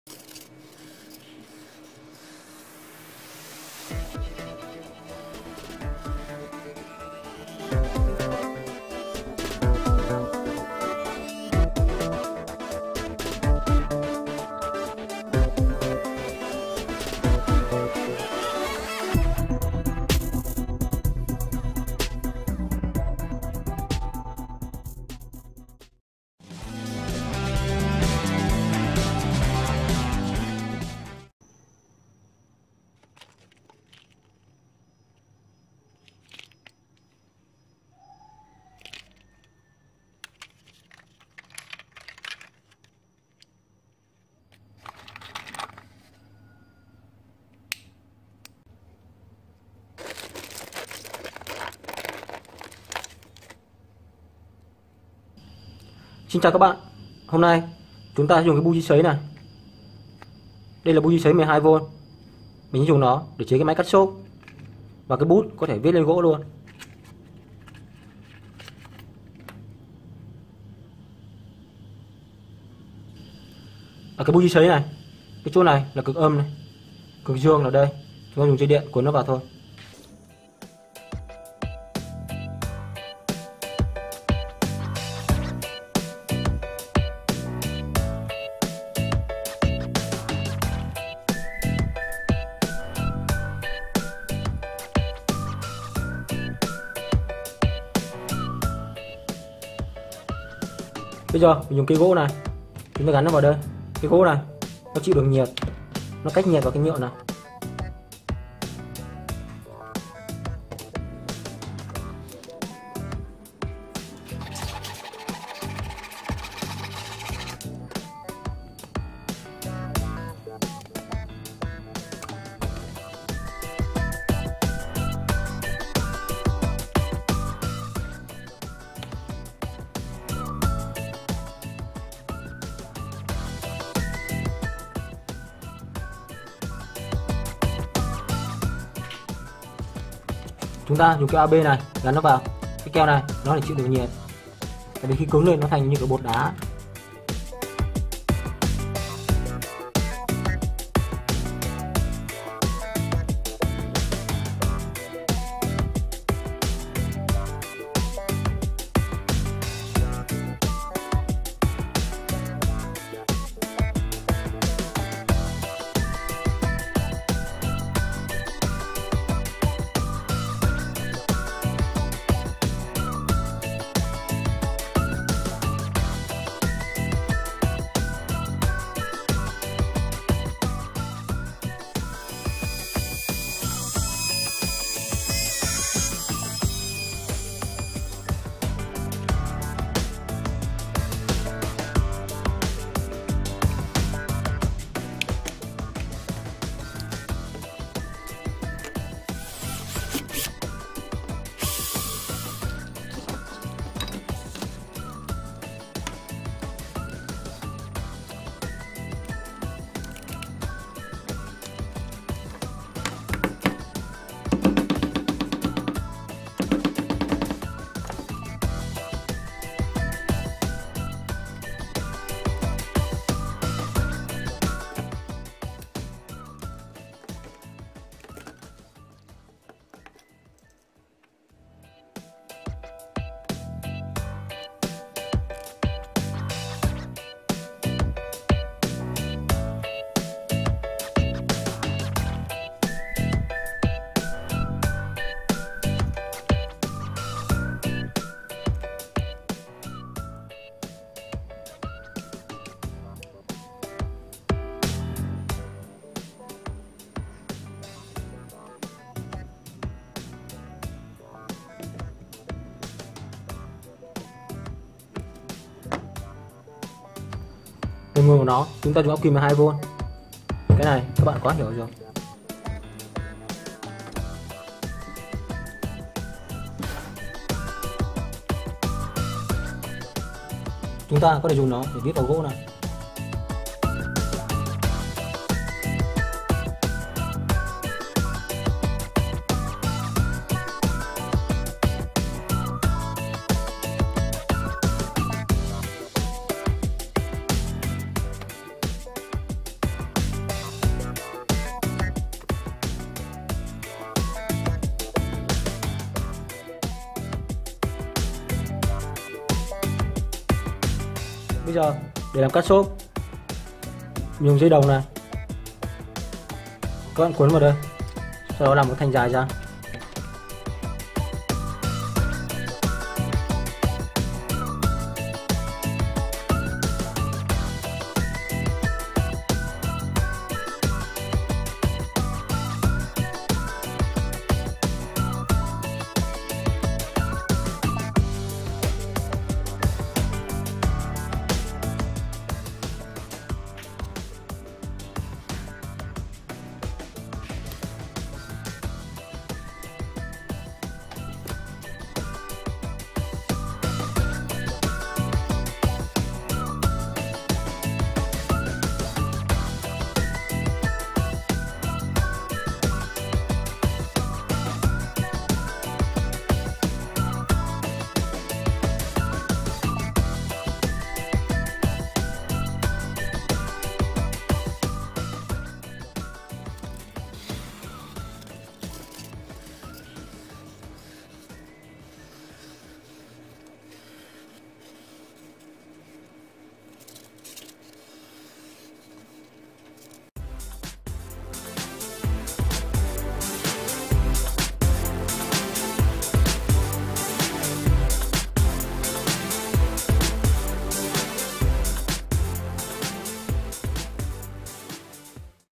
Chế BÚT LỬA viết lên gỗ, Bút Cắt Xốp 2 in 1 từ Bugi Sấy
Hôm nay chúng ta sẽ cùng Chế BÚT LỬA viết lên gỗ, Bút Cắt Xốp 2 in 1 từ Bugi Sấy
Kênh Sáng Tạo .COM Chúc các bạn thành công !!!